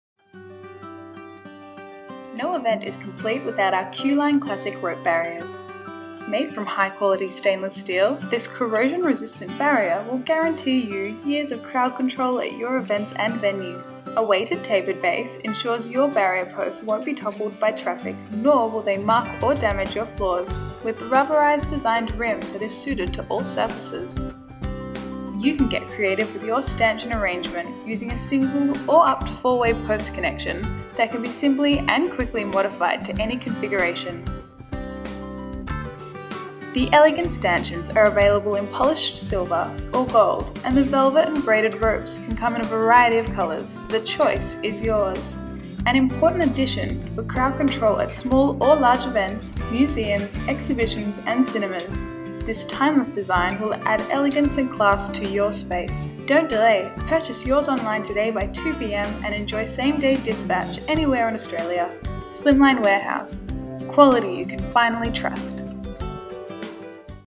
Classic Silver & Gold Barrier Post - Quality Demonstration
Crowd Control Barriers
These classic stanchion posts are extremely durable and hard wearing units. Constructed from high quality stainless steel, the safety barriers are coated in titanium and polished.
The base of this rope barrier weighs 7kg, making them very hard to topple and ensuring great stability even when used with a number of ropes attached to the top of the stanchions.
These stylish barrier posts are designed for use with our Q-Line velvet ropes which are available individually in Black, Blue & Red.
This crowd control system is available in 2 finishes.
Video includes happy acoustic and/or soft instrumental music for your enjoyment.